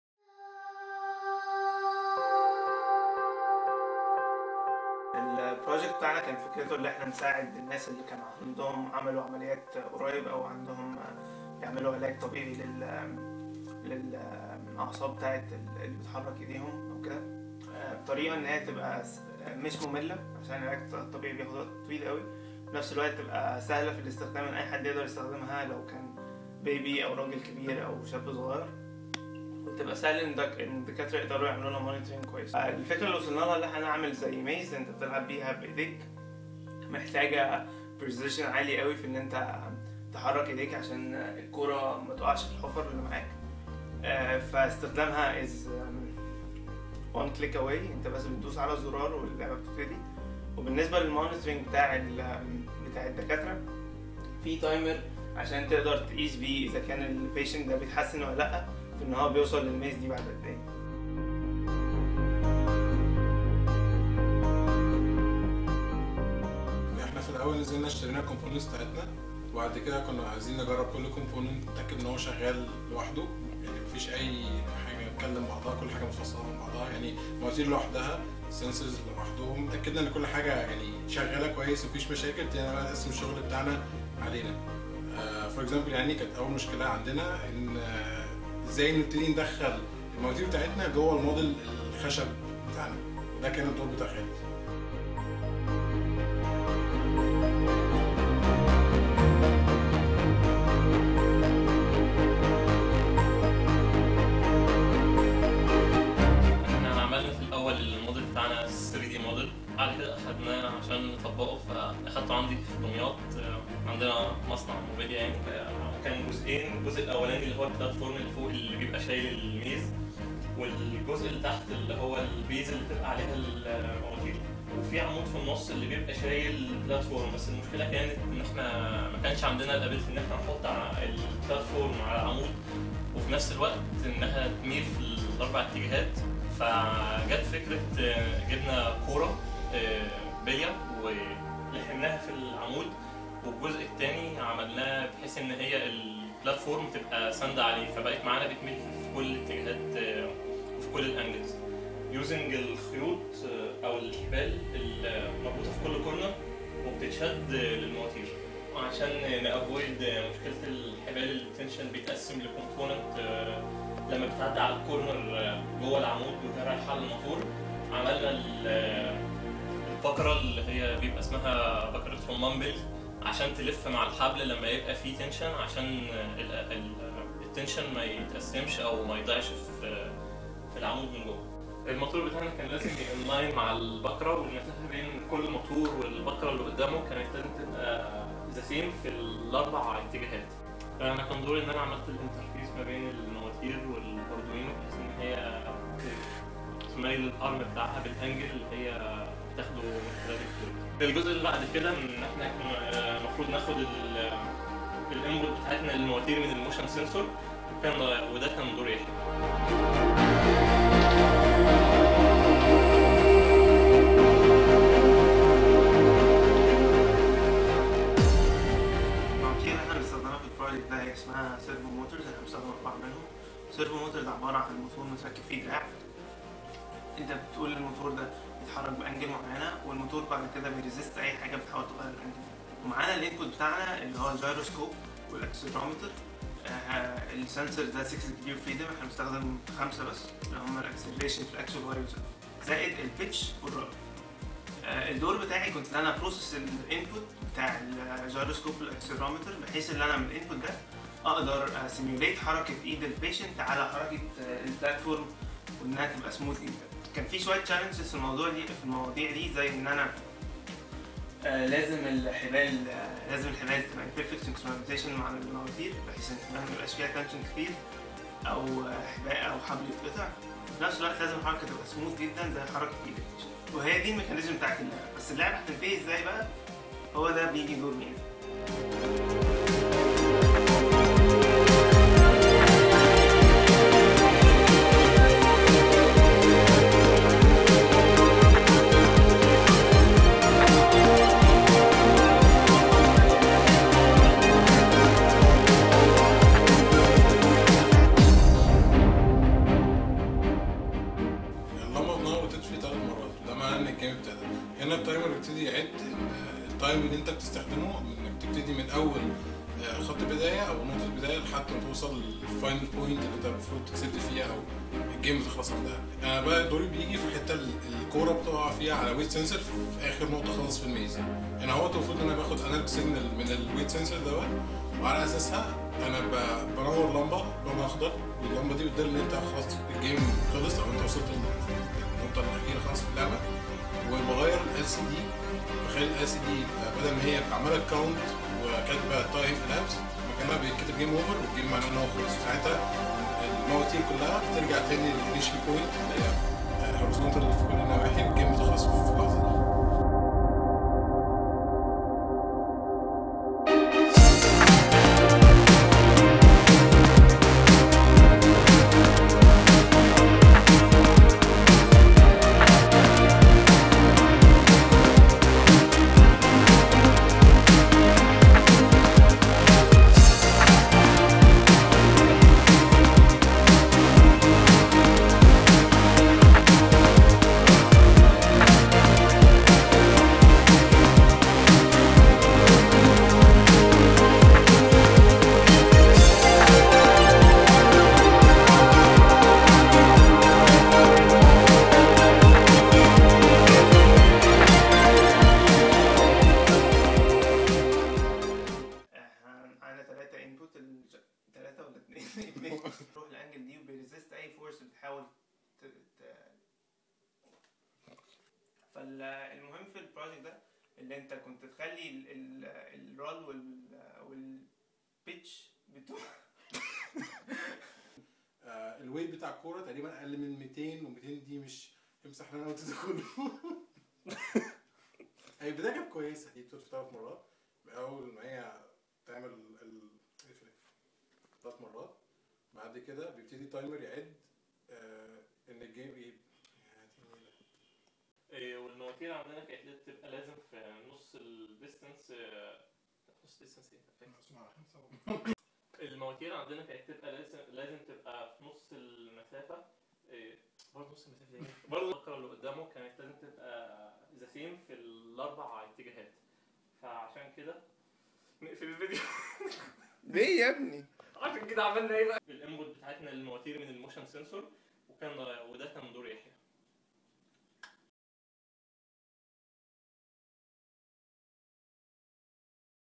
T7_csen701project (Labyrinth)
Our idea was to create a device that can help patients that are recently had surgical operation related to nervous system or recovering from a disease that had limited their motor nerfs to do their tasks.These patients need a special type of treatment like physiotherapy for long time to be able to gain their original natural movement and that mean that they need doctors to follow their progress with time and decide weither the patient is responding or not.So here came our idea,our criteria of choosing this device was that we want it to be:1)Easy in usage.2)Fun for the patient so that long term therapy doesn't become boring.3)Easy to be monitored by Doctors and noticing weither the patient state is improving or not.The only thing that matched all these criteria was an old game called Labyrinth.

Project Made By:
Khaled El-Bahaey
Mina Essam Saad
Yahia El-Gamal